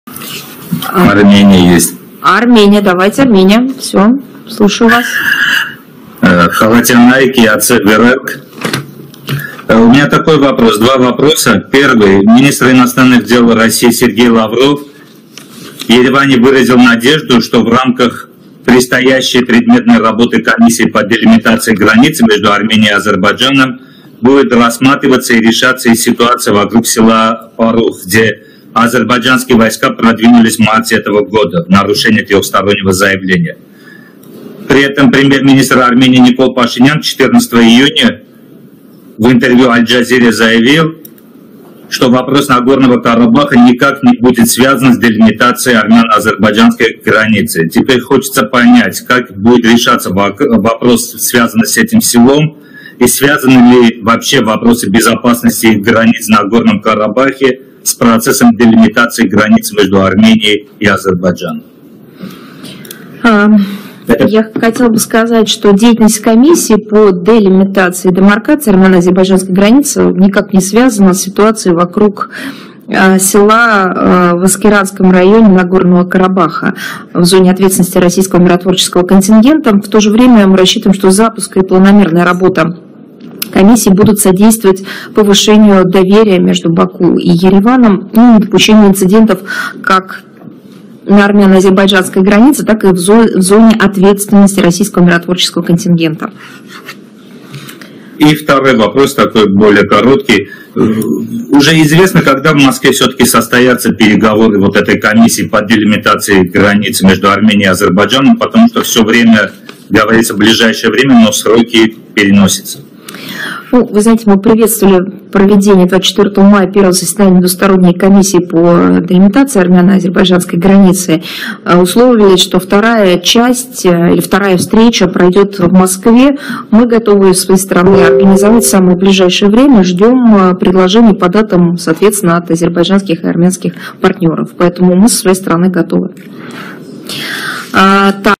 Սահմանազատման և սահմանագծման հանձնաժողովի գործունեությունը կապ չունի Փառուխի շուրջ իրավիճակի հետ
Հայ-ադրբեջանական սահմանի սահմանազատման և սահմանագծման հանձնաժողովի գործունեությունը ոչ մի կապ չունի ռուսական խաղաղապահ զորախմբի պատասխանատվության գոտում գտնվող Լեռնային Ղարաբաղի Ասկերանի շրջանի Փառուխ գյուղի շուրջ ստեղծված իրավիճակի հետ։ Այդ մասին ամենշաբաթյա ճեպազրույցի ժամանակ հայտարարել է ՌԴ ԱԳՆ պաշտոնական ներկայացուցիչ Մարիա Զախարովան։

«Միևնույն ժամանակ մենք ակնկալում ենք, որ հանձնաժողովի մեկնարկն ու լայնածավալ աշխատանքը կնպաստեն Բաքվի և Երևանի միջև վստահության բարձրացմանն ու միջադեպերի կանխմանը՝ ինչպես հայ-ադրբեջանական սահմանին, այնպես էլ ռուս խաղաղապահների պատասխանատվության գոտում»,- ասել է Զախարովան։

Հայ-ադրբեջանական սահմանի սահմանազատման և սահմանագծման հանձնաժողովի երկրորդ նիստի ժամկետի մասին հարցին Զախարովան պատասխանել է.

«Մենք ողջունեցինք մայիսի 24-ին հայ-ադրբեջանական սահմանի սահմանազատման հարցերով երկկողմ հանձնաժողովի առաջին նիստի անցկացումը, պայմանավորվեցինք, որ երկրորդ մասը կամ հանդիպումը տեղի կունենա Մոսկվայում։ Մենք պատրաստ ենք այն կազմակերպել շատ մոտ ապագայում, սպասում ենք հայ և ադրբեջանցի գործընկերների առաջարկներին»։